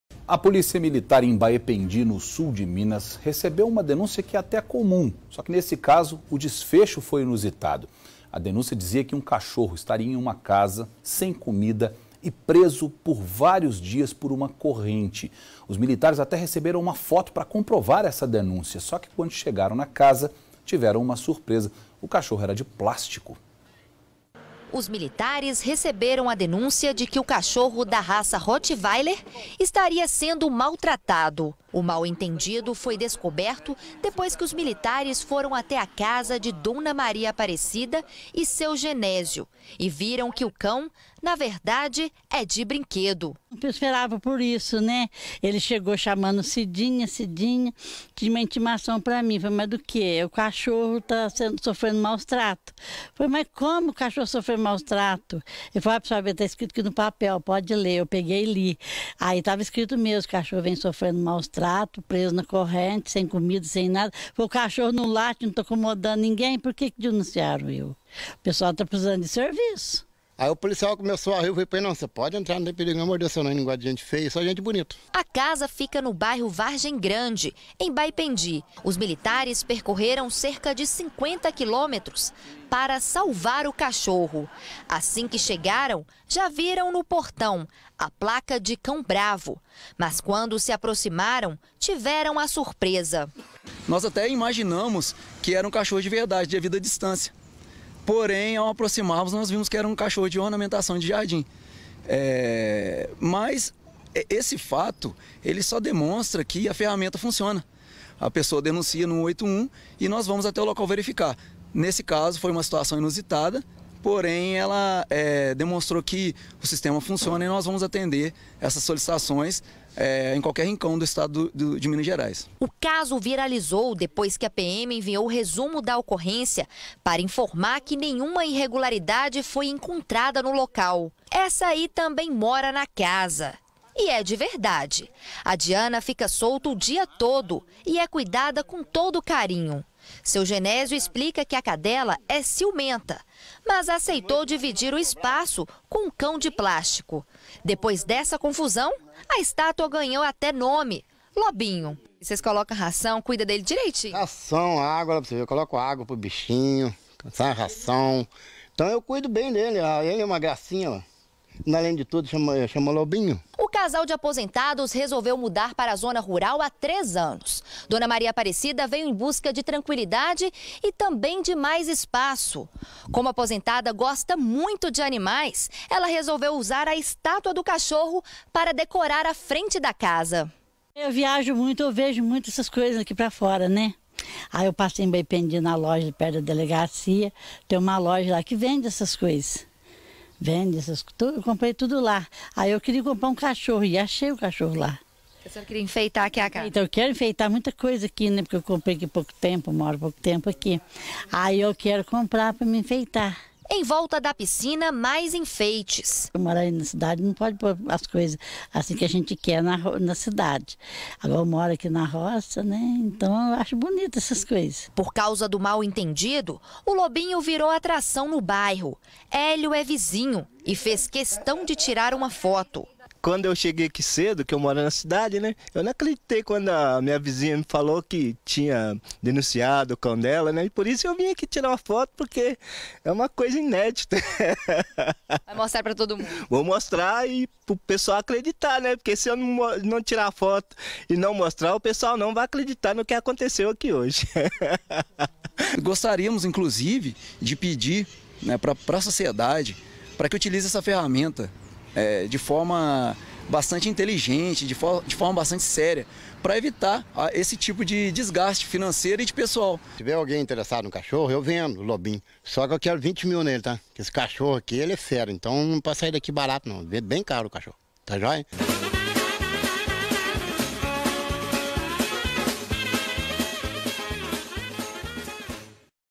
Maus-tratos a cão: policiais vão checar e são surpreendidos
A Polícia Militar, em Baependi, no Sul de Minas, recebeu uma denúncia dizendo que um cão estaria em uma casa, sem comida e preso, por vários dias, em uma corrente. Os militares até receberam uma foto para comprovar a denúncia. Só que, quando chegaram na casa, tiveram uma surpresa: o cachorro era de plástico.